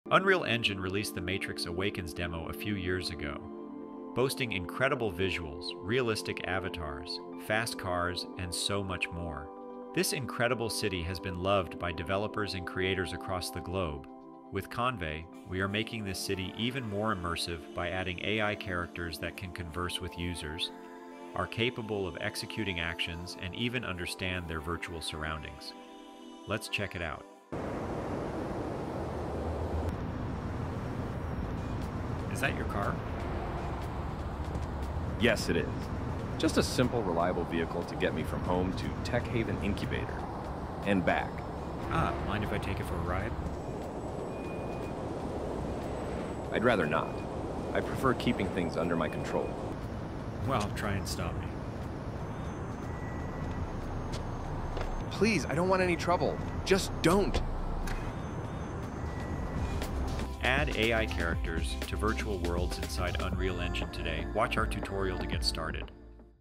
Unleash AI Characters into The Matrix! 🚀
At Convai, we enable developers to create realistic AI characters, which can interact, adapt, and understand the world around them. By adding AI into characters within richly detailed environments such as the City Sample project by Epic Games, the surroundings feel more engaging and lively! These characters gain spatial awareness, social interaction capabilities with both humans and other AIs, and the agency to pursue goals, objectives, and even memories—leading to lifelike behaviors and social skills.
In our detailed tutorial, we dive into transforming hundreds of static entities with the expansive City Sample project (Matrix Awakens) into dynamic, AI-powered NPCs in Unreal Engine. Our step-by-step guide will help you integrate AI into these characters, empowering them with spatial awareness, interaction skills, evolving objectives, and memories that enable persistent virtual worlds. Join us as we dive into blueprints, tackle the complexities of Unreal Engine AI NPCs, and bring AI-powered characters to life in a Matrix-inspired simulation!